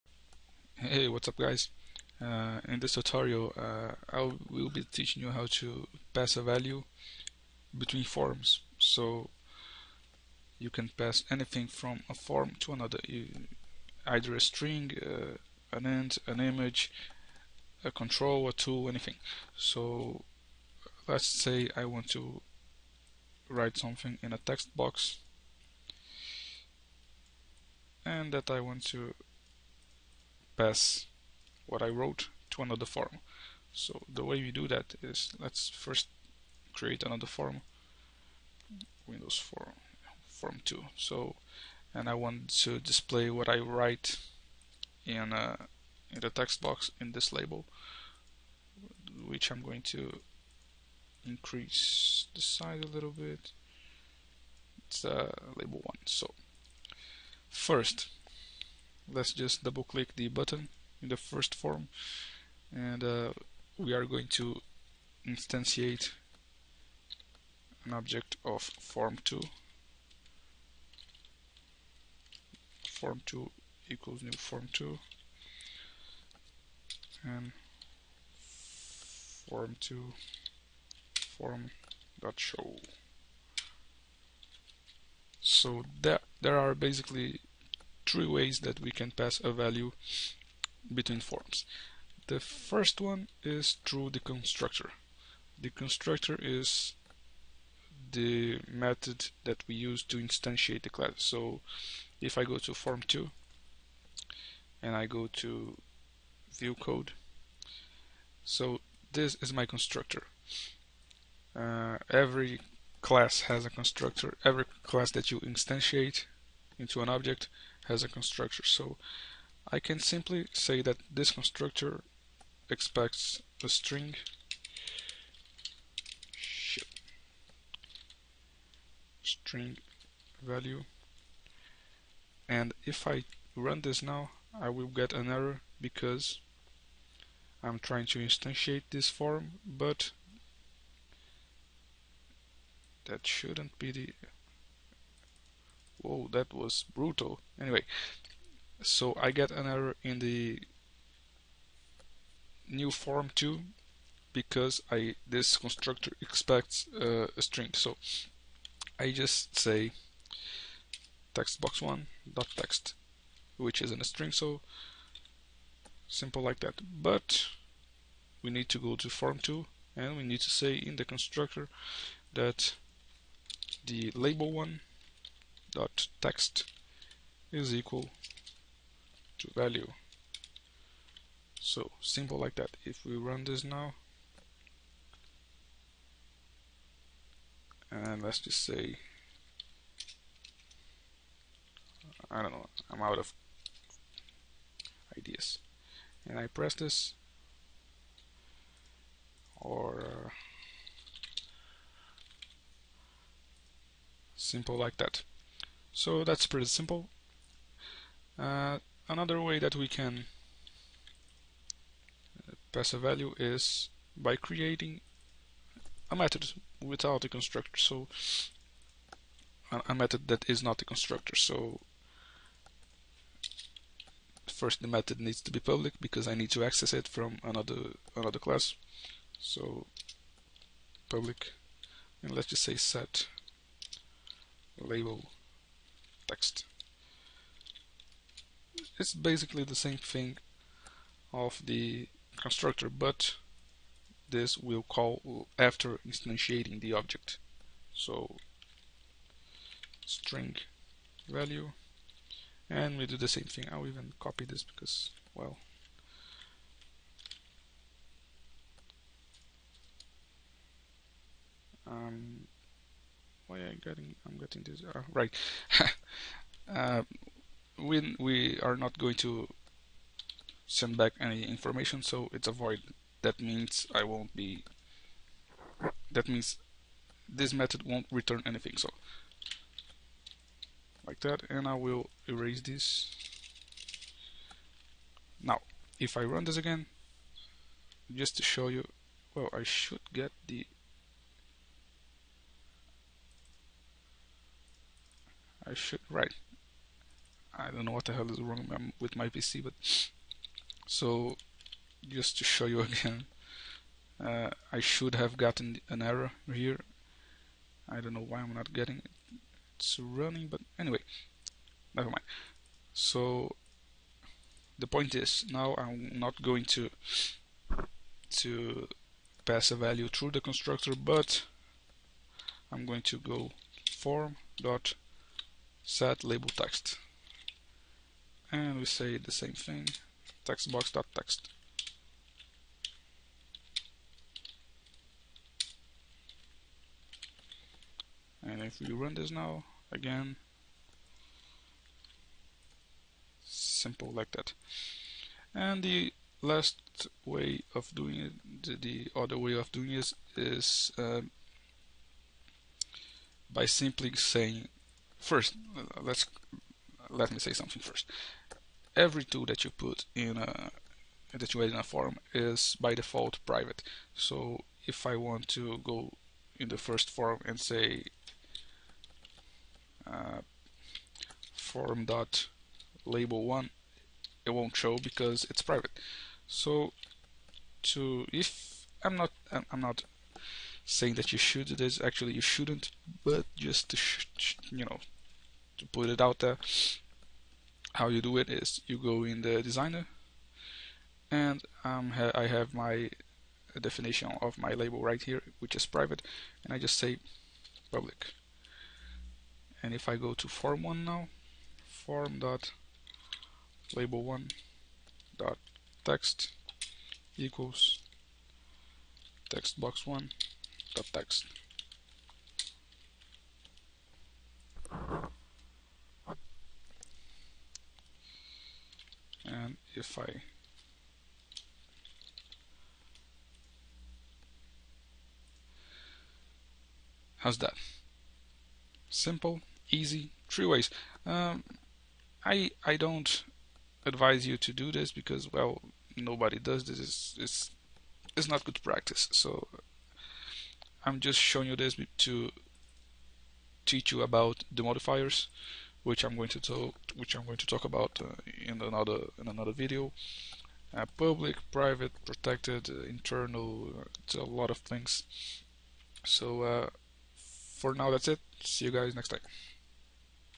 C# Tutorial - Passing values between forms
Learn how to pass values through forms in three diferent ways.